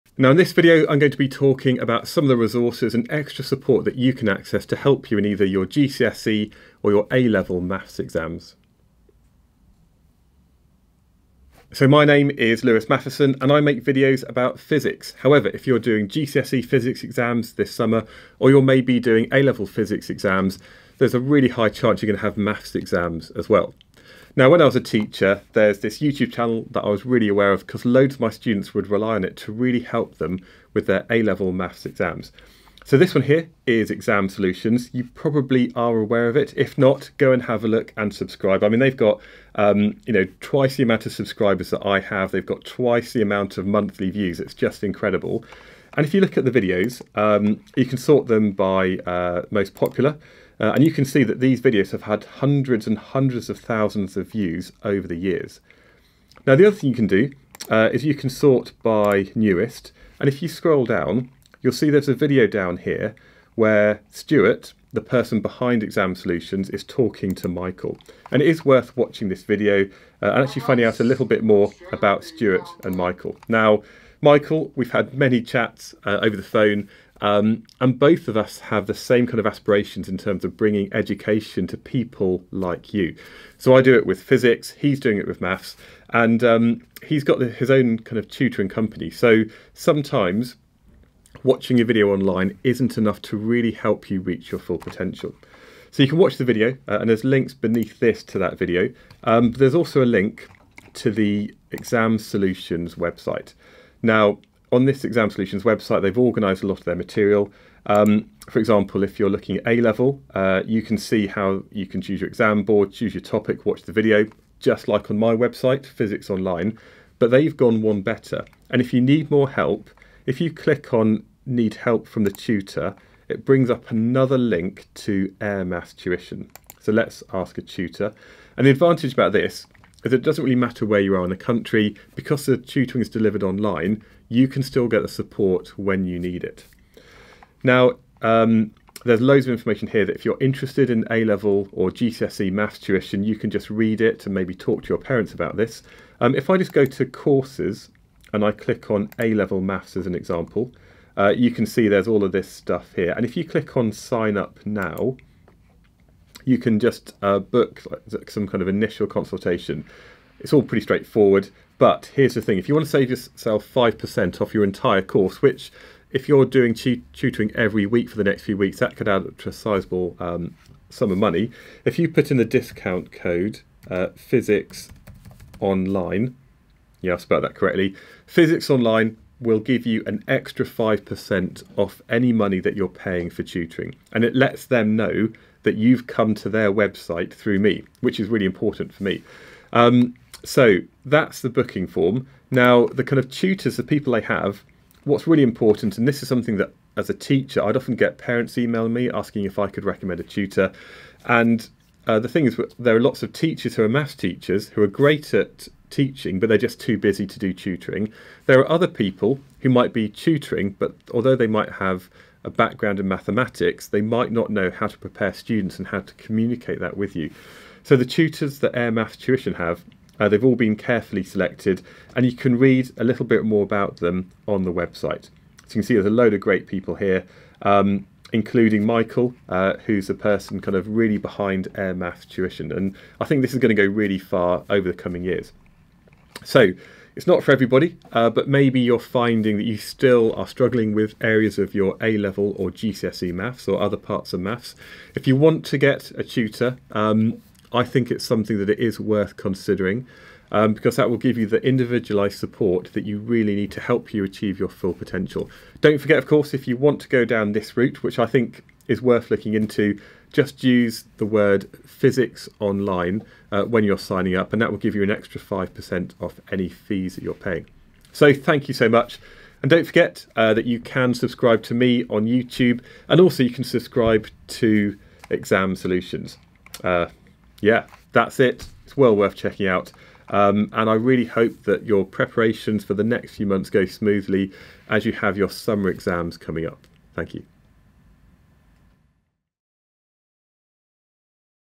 Want the HIGHEST Grades in GCSE and A Level Maths? Exam Solutions and Air Maths 2022
Do you want the highest grades in GCSE and A Level maths? If you are considering tutor support to help you with your A Level or GCSE Maths, then Exam Solutions is an amazing YouTube channel that you should know about - now working with Air Maths Tuition to provide individual support. and use the discount code 'physicsonline'